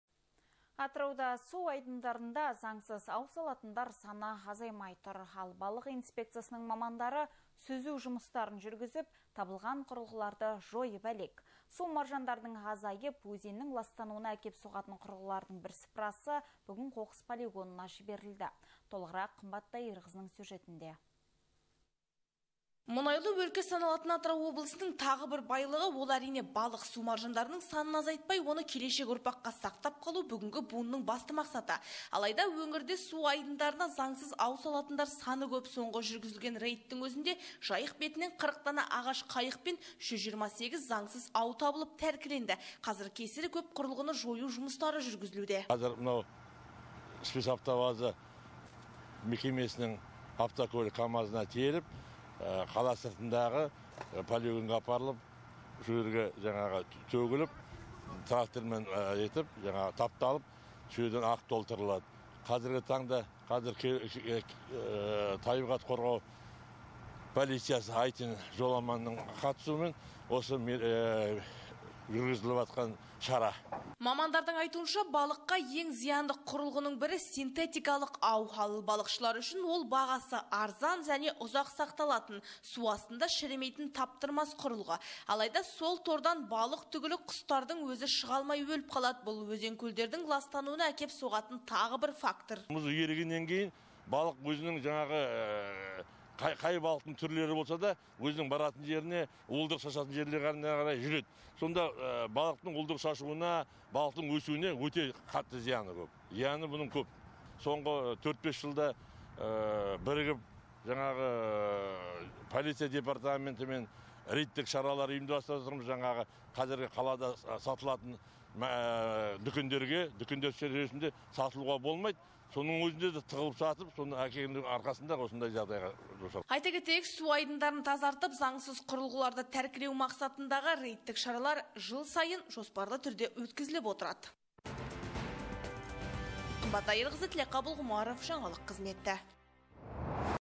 ЗАҢСЫЗ АУ САЛАТЫНДАР КӨП
Атырауда су айдындарына заңсыз ау салатындар саны азаймай тұр.  Ал балық инспекциясының мамандары  сүзу жұмыстарын жүргізіп, табылған құрылғыларды жойып әлек. Су маржандарының азайып, өзеннің ластануына әкеп соғатын құрылғылардың бірсыпырасы бүгін қоқыс полигонына жіберілді. Толығырақ Қымбат Даирқызының сюжетінде.